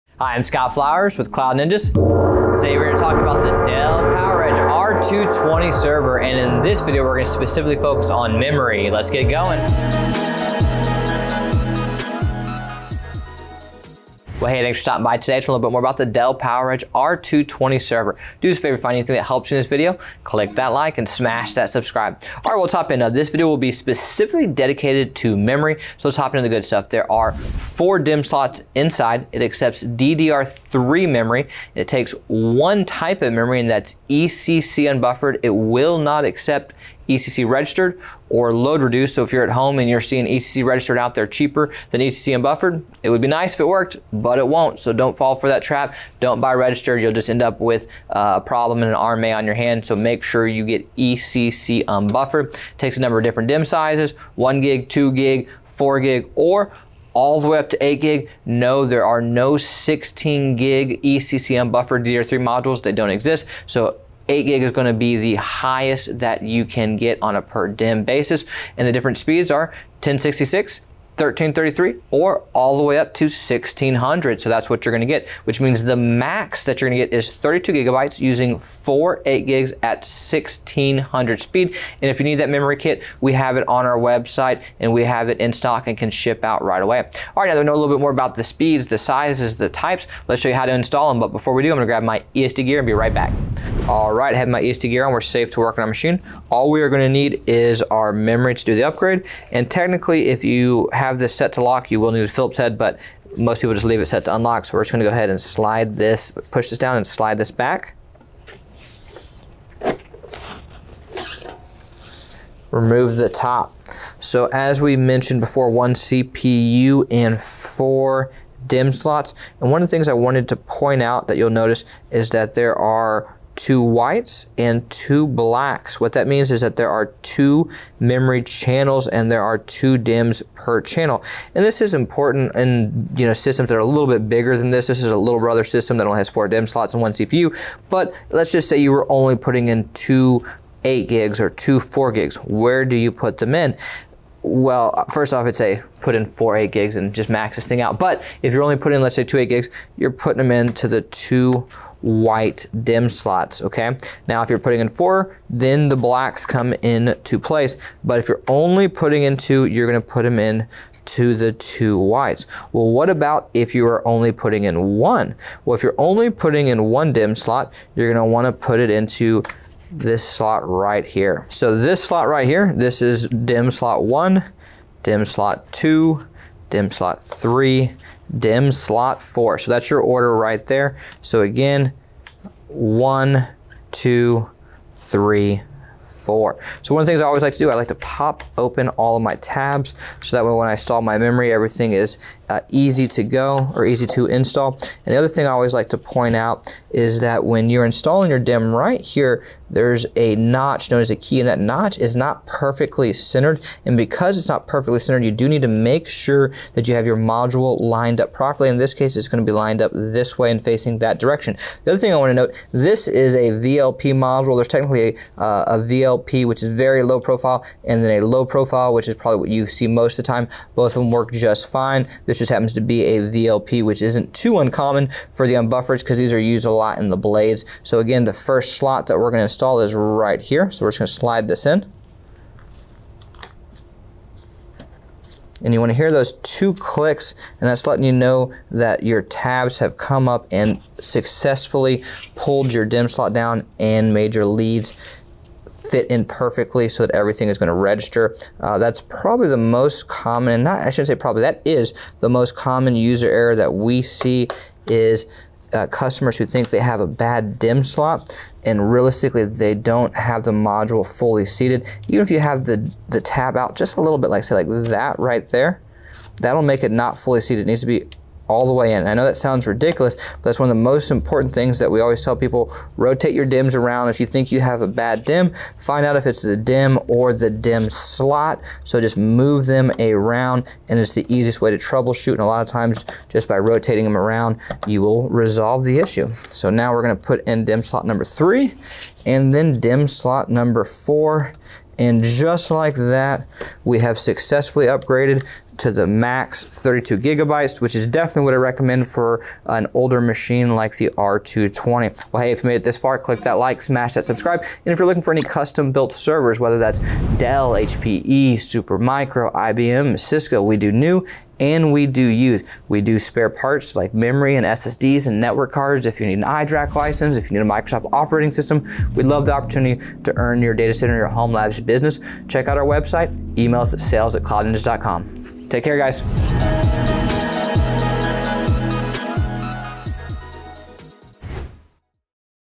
Dell PowerEdge R220 Server Memory Spec Overview & Upgrade Tips | How to Configure & Install DDR3 RAM
Welcome to our video on revitalizing your Dell PowerEdge R220 by exploring its memory options and configurations. In this guide, we'll show you how to install memory modules to maximize your server's performance.

We value your feedback! Our goal is to make this video series a valuable resource for the Dell community.
The Dell PowerEdge R220 supports DDR3 memory with 4 DIMM slots, accommodating only ECC Unbuffered modules. You can install 1GB, 2GB, 4GB, or 8GB DIMMs at speeds of 1066MT/s, 1333MT/s, or 1600MT/s. The maximum capacity is 32GB, achieved by installing four 8GB modules at 1600MT/s.

Join us as we dive into optimizing the memory capabilities of your Dell PowerEdge R220!

Don’t miss on our Deals of The Week for a chance to purchase a server at a competitive price: